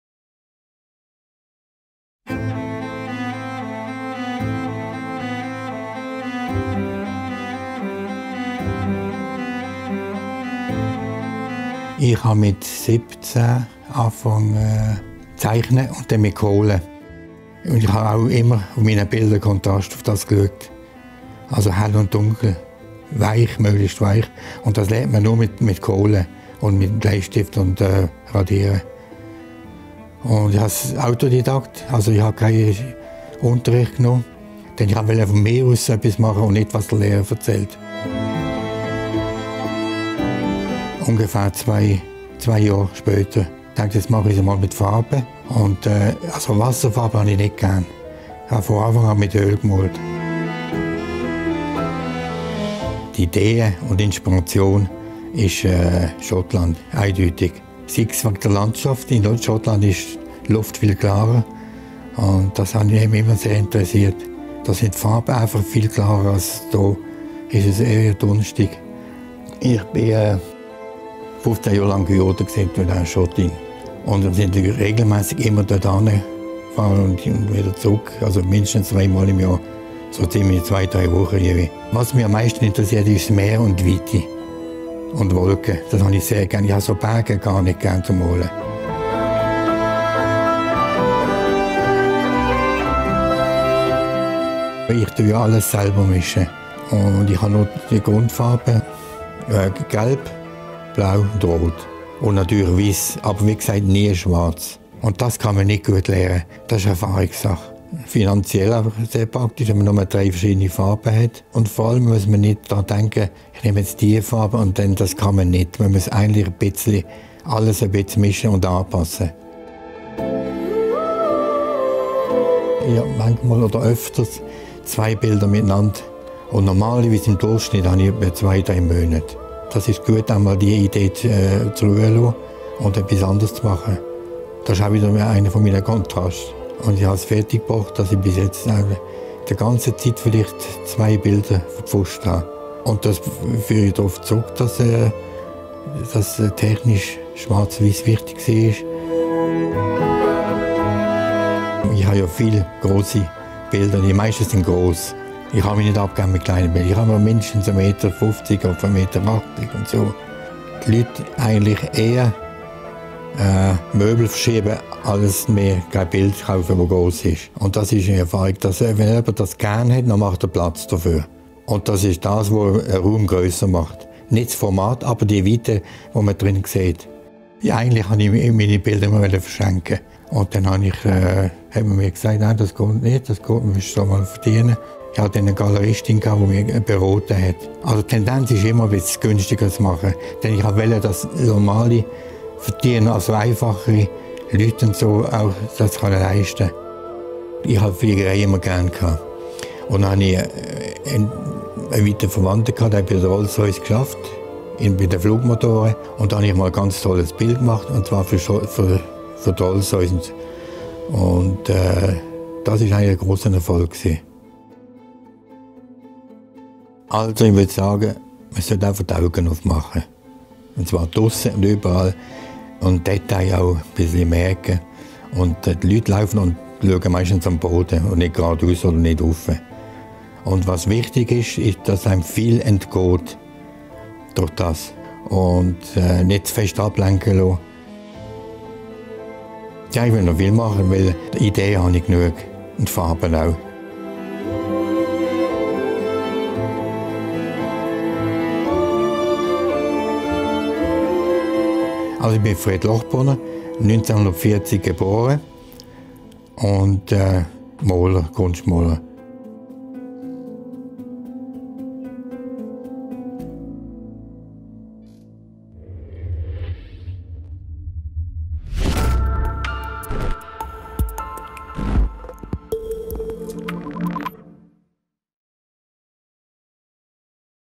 Portrait eines Malers – Fred Lochbrunner
Portrait über den Basler Kunstmaler Fred Lochbrunner.